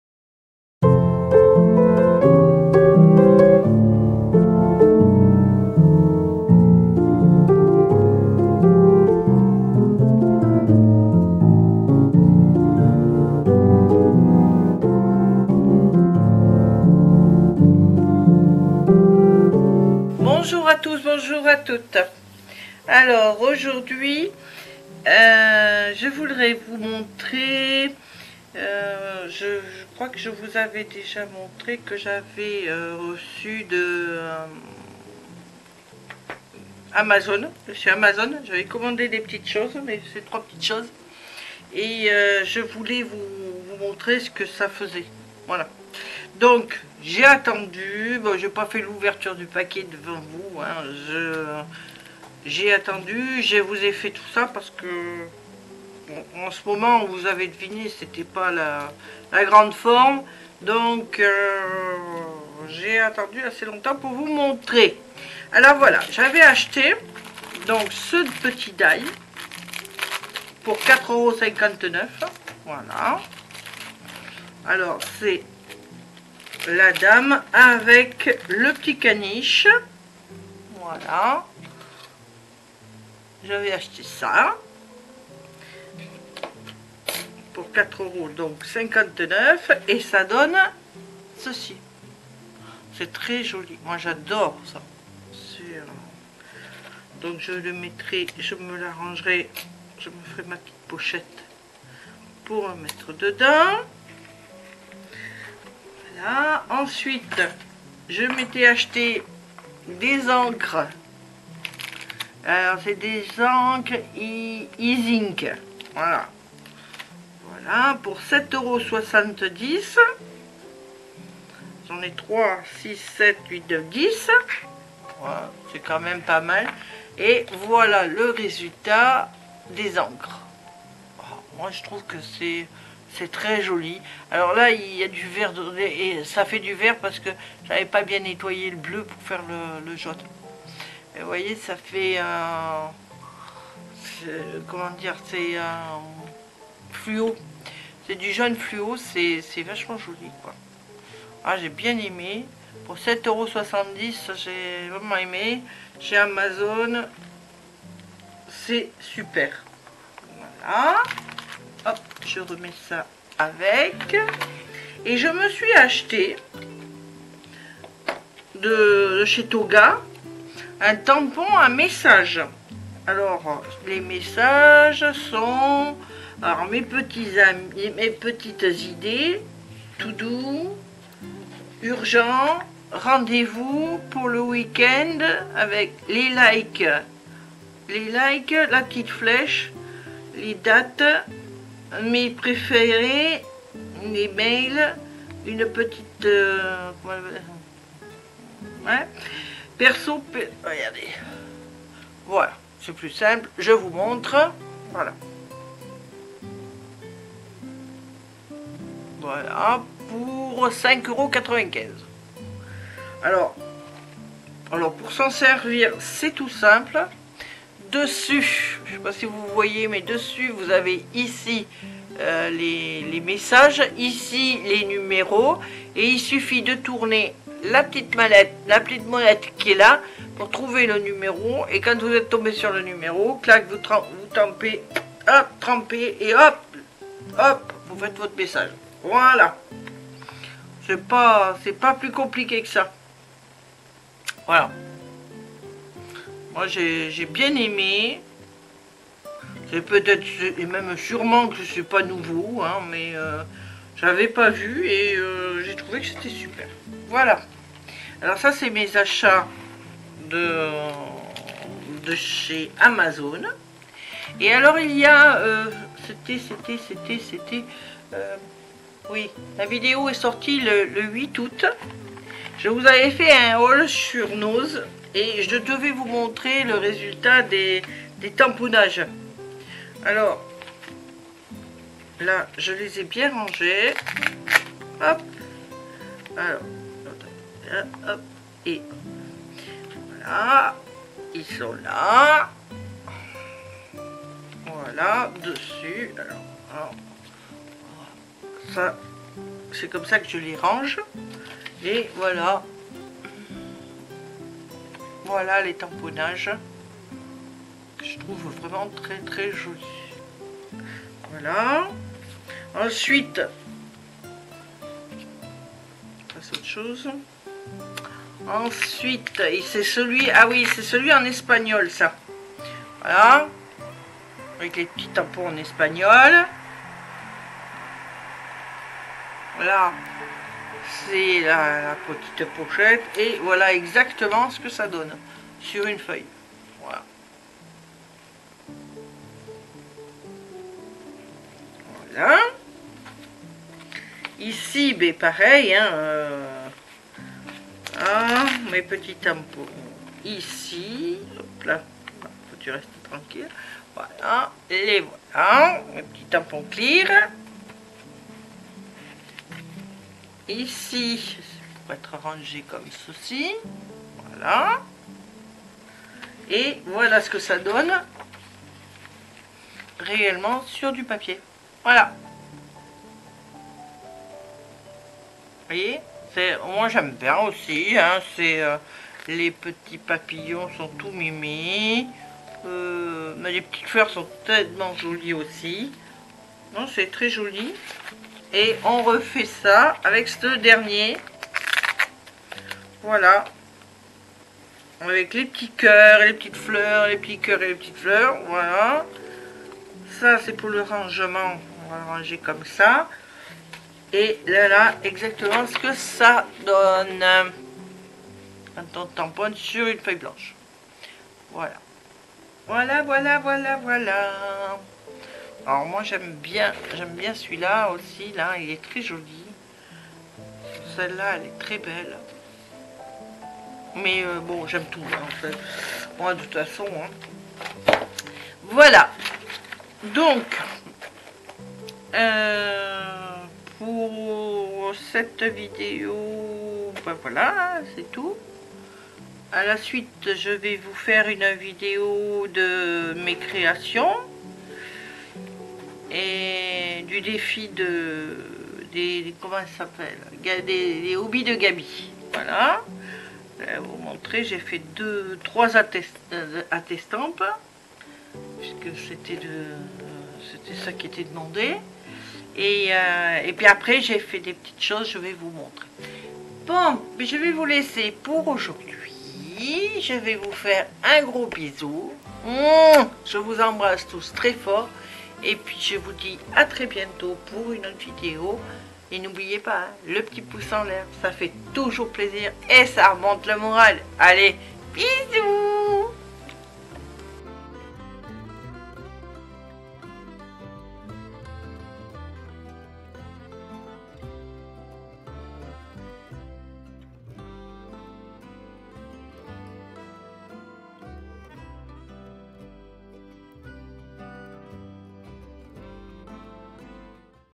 Haul amazone avec resultat tampons Noz
dans cette video voici mes dernieres achats amazone ainsi que comme vous l attendiez depuis le 8 aout les resultats sur papier des tampon clears acheter chez noze
gros bisous

musique
je vous donne le lien par lequel vous pouvez essayer gratuitement pendant 30j